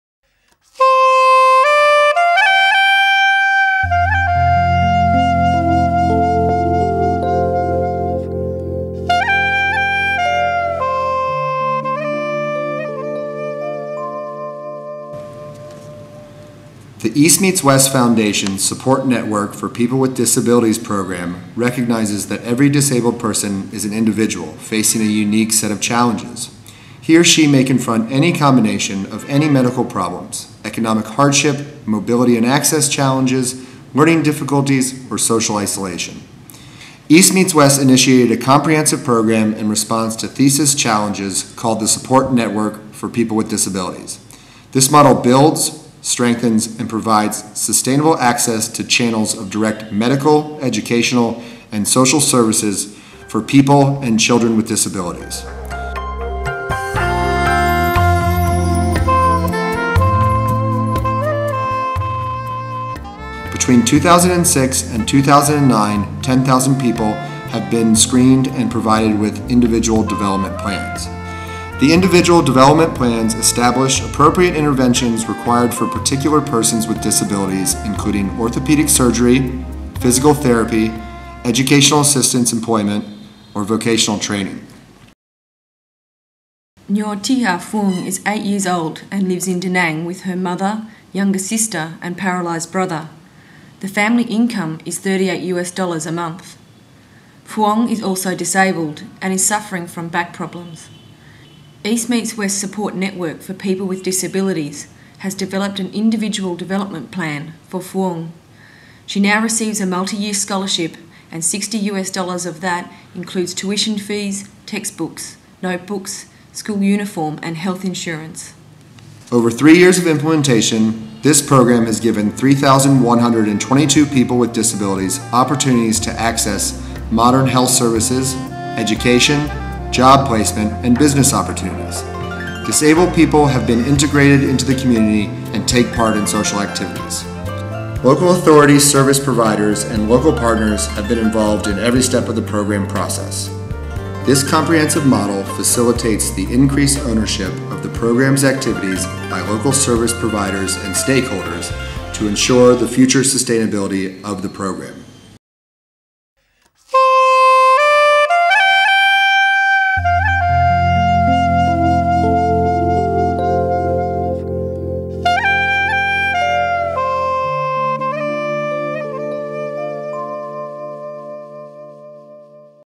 Support Network for People with Disability _ The program model of EMW
East Meets West Foundation's Support Network for People with Disabilities (SN-PWD) creates channels of medical, educational and job training services for people with disabilities in Vietnam.
SN-PWD was originally launched in Quang Ngai Province with a grant from the Ford Foundation and has now expanded to Thai Binh and Da Nang with various grants from international donors such as USAID and IrishAid. All three areas have a large population living with disabilities, with a high percentage suspected to relate to dioxin exposure.
The SN-PWD program recognizes that every disabled person is an individual, facing a unique set of challenges. Disabled people may confront any combination of medical problems, economic hardship, mobility and access challenges, learning difficulties or social isolation. SN-PWD starts with individual medical and social screening to determine the needs of each disabled individual. Depending on those needs, the program provides: corrective surgeries, including surgery for children with congenital heart defects; educational assistance through a multi-year scholarship program; physical therapy and rehabilitation, either home based or in locally accessible neighborhood centers; vocational training and job placement assistance; assistive equipment, such as wheelchairs and prosthetic devices.
The SN-PWD program works to increase the availability of services for people with disabilities, including expanding the capacity of health care facilities to provide rehabilitation, improving the skills of local health care providers, and facilitating the creation of employment opportunities. Capacity building activities include: establishing community based rehabilitation centers; providing technical support and physical therapy/rehabilitation devices to the se centers; strengthening the strategic planning and implementation capacity of EMW's local strategic partners; and, improving the skills of medical personnel treating people with disabilities.
Through all these channels, SN-PWD aims to assists people with disabilities to access locally available, effective support so they can live healthier, longer, more prosperous lives, enriched by greater options.